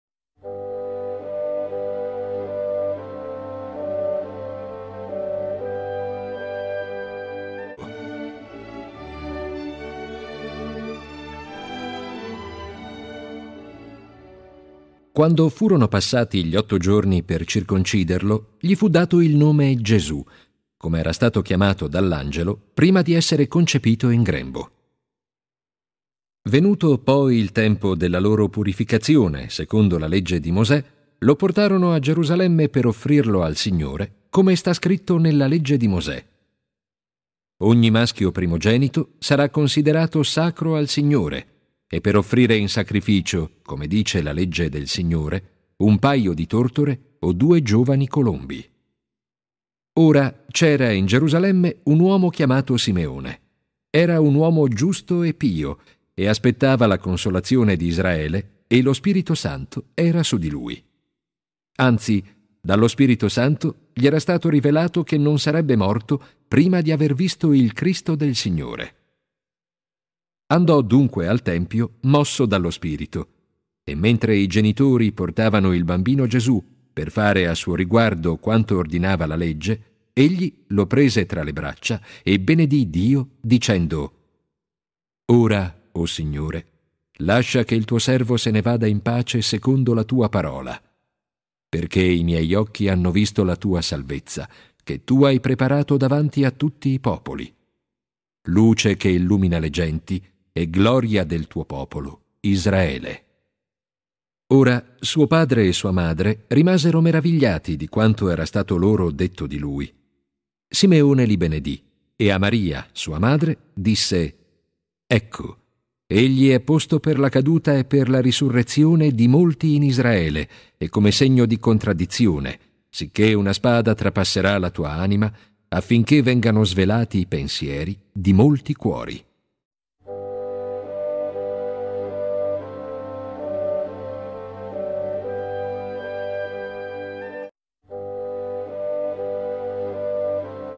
CIRCONCISIONE E PRESENTAZIONE AL TEMPIO S.Lc 2,22-35
La circoncisione che era per gli ebrei il nostro battesimo, Gesù la ricevette non per bisogno o obbligo, ma per rendersi simile si fratelli e per redimere anche quelli che erano sotto la Legge.
Gesù è detto Messia, o Cristo,o Unto , perché è re, profeta e sacerdote, e questi erano consacrati con la sacra unzione. Gesù venuto per salvare tutti, divenne pietra di scandalo per Israele (Is 8,14; Rom9,32).
Simeone predice la passione, che come una spada trafiggerà il cuore di Maria, la quale, sebbene sapesse che Gesù doveva morire spontaneamente, pure come madre, non poteva ciò sopportare senza spasimo.
Tela: 65x45cm.
Tecnica: Acrilico su tela
Anno: 2020
Bibbia di Gerusalemme
IL VANGELO
Direzione: Carlo D’Angelo